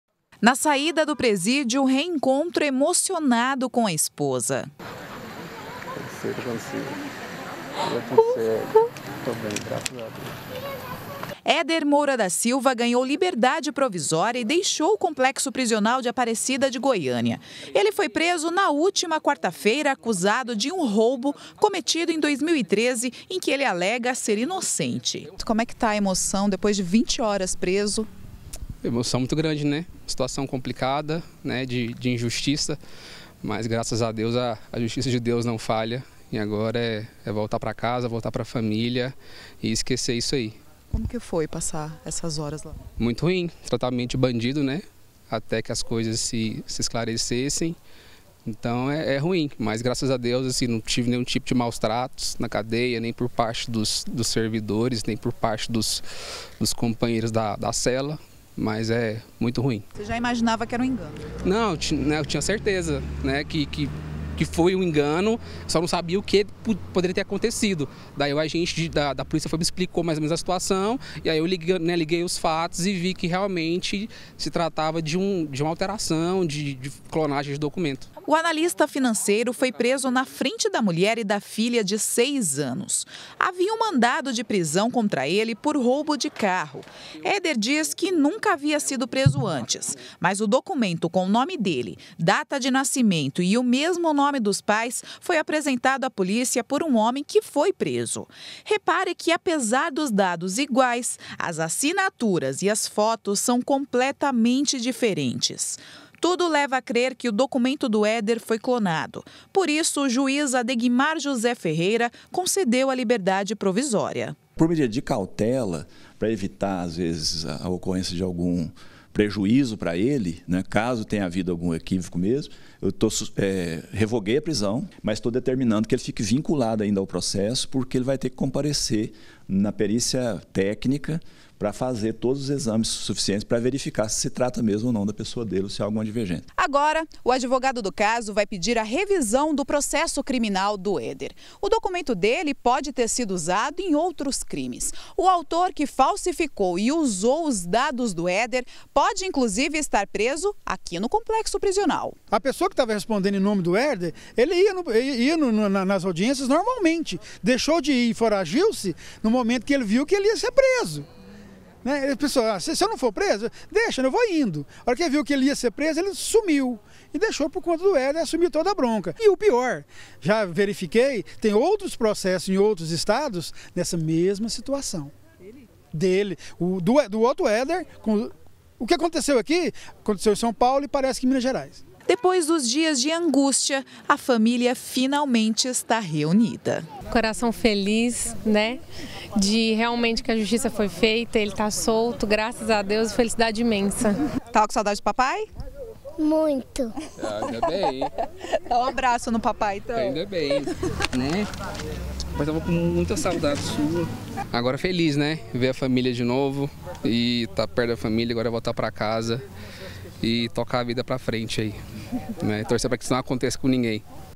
PRESO INJUSTAMENTE: HOMEM RECEBE LIBERDADE PROVISÓRIA
Desde o início da semana você tem acompanhado aqui na Record TV , o sofrimento de um analista financeiro que foi preso injustamente. Recebeu a liberdade provisória ontem à noite. A repórter Adriana de Luca acompanhou o momento em que o homem reencontrou a família.